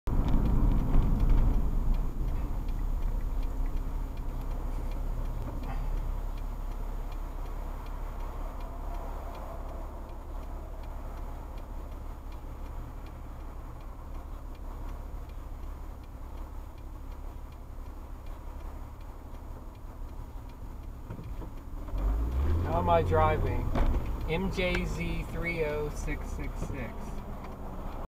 Impatient FSNS driver moves to left of car to turn right in front of it
MJZ3606